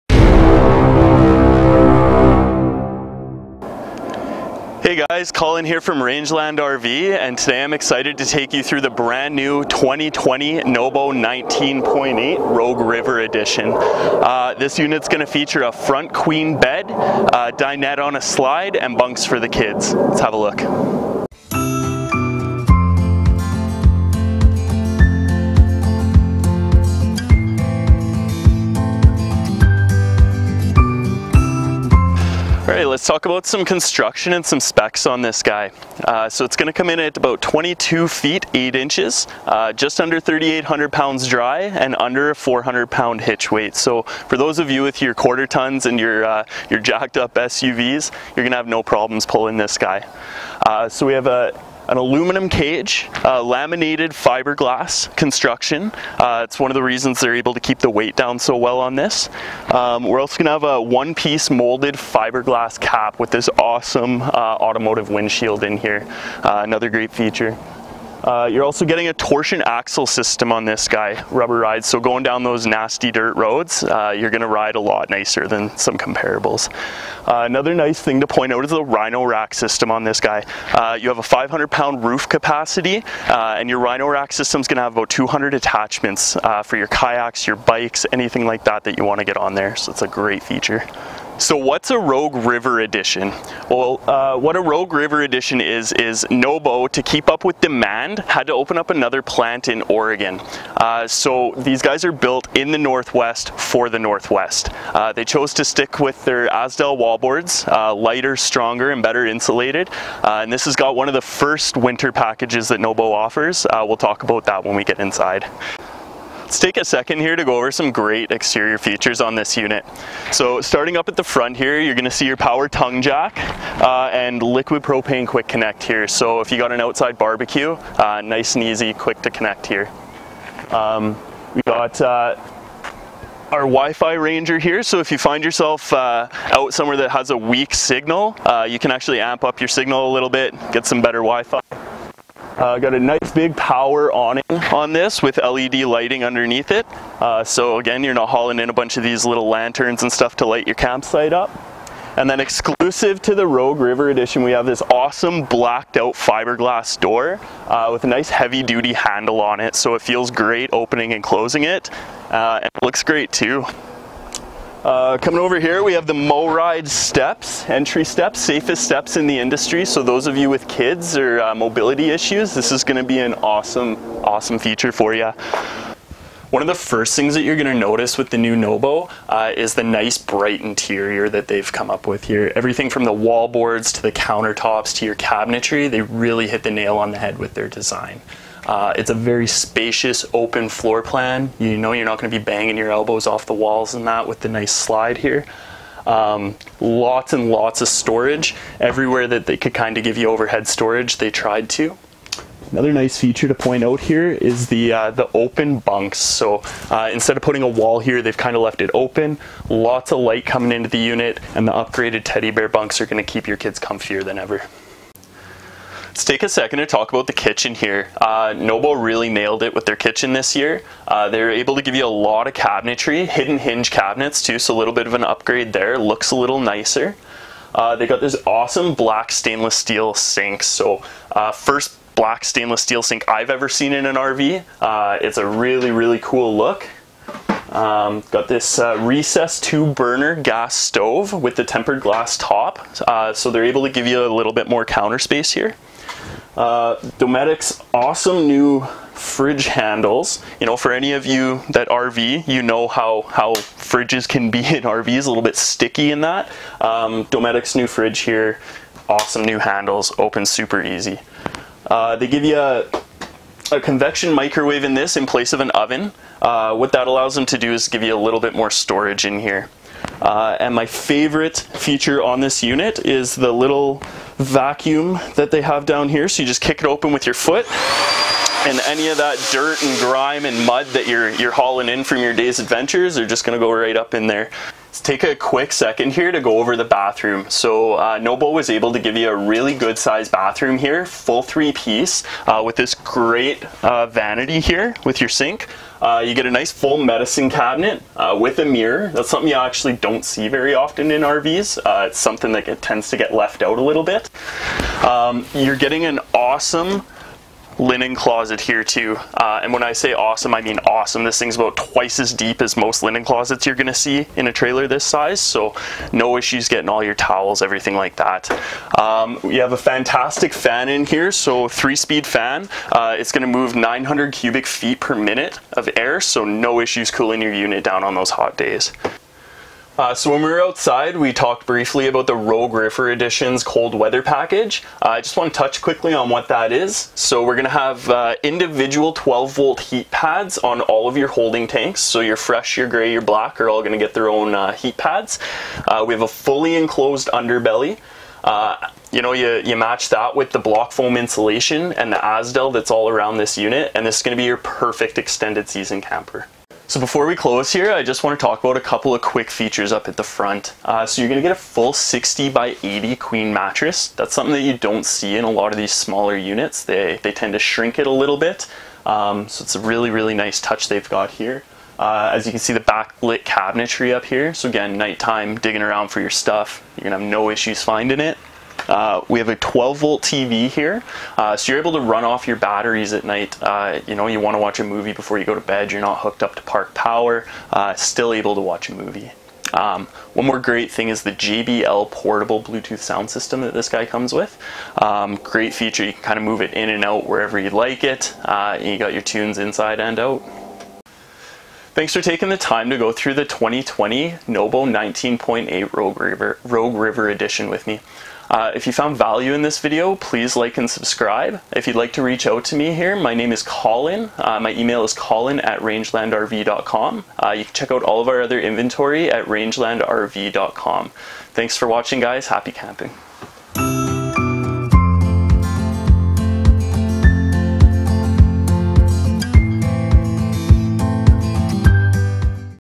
2020 NoBo, No Boundries, 19.8 Rogue River, Bunk Beds, walk through, Rangeland RV.
Watch a great video on the all new 2020 NoBo Rogue River Edition with Collin "The RV Guy," of Rangeland RV. Collin talks about construction and all of the new features available in this very informative video. Find out why No Boundries 19.8 might just be the best off road trailer in the RV industry today!